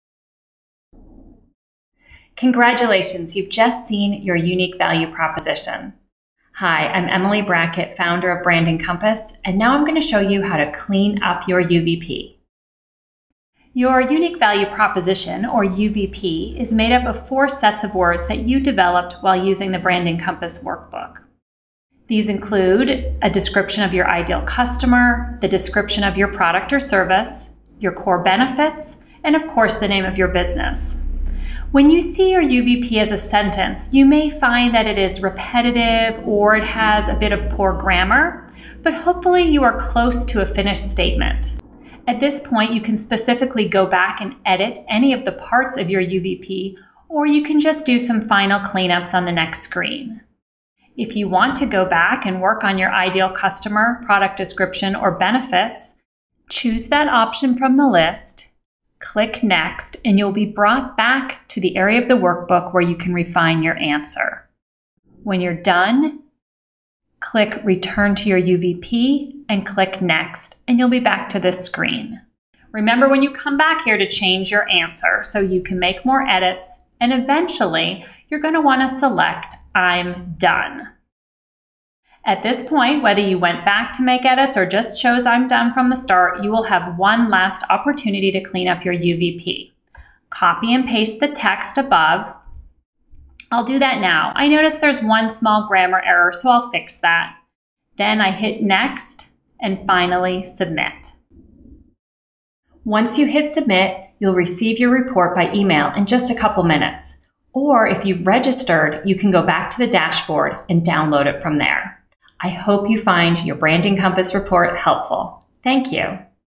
Write and Edit Your Unique Value Proposition with Branding Compass
Congratulations! You’ve seen your UVP or unique value proposition that has been automatically created by Branding Compass.

Your UVP is made up of four sets of words that you developed while using the Branding Compass workbook. These include:
A succinct description of your your ideal customer
The description of your product or service
The core benefits of your product or service
And of course the name of your business.

When you see your UVP as a sentence you may find that it is repetitive, or has a bit of poor grammar. But hopefully you are close to finished statement. At this point you can specifically go back and edit any of the parts of your UVP, or can just make some final edits on the next screen.

If you want to go back and edit your ideal customer, your product description, or your benefits, choose that option from the list. Make your changes and then click Return to your UVP to get back to this screen. Remember when you come back here to change your answer so you can do more edits, and eventually to select I’m done.

At this point, whether you went back into your workbook to make edits, or just chose I’m done right from the start, you will have one last opportunity to clean up your UVP.

So here I am, I’m going to copy and paste the text. I notice that there is one small grammar error so I’ll fix that. Then hit next and finally submit.

Once you hit submit you should receive your report by email in just a couple of minutes. If you registered your account you can also login to your dashboard and download your report from there.